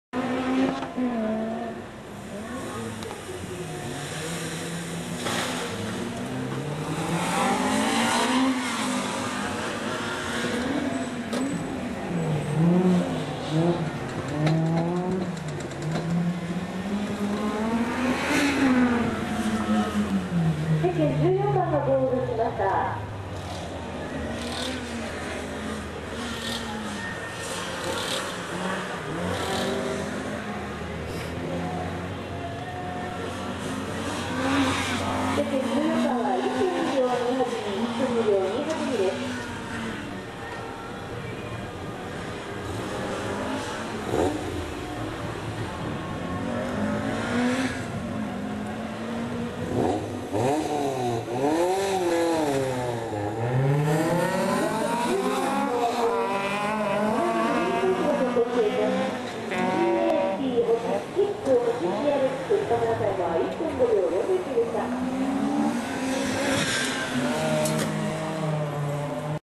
2007/6/17 CAP！☆SKIP！☆CR-X 第2ヒート
2007/6/17JAF東北ジムカーナ選手権第4戦SA1クラス第2ヒート
車両名:CAP！☆SKIP！☆CR-X
場所:仙台ハイランド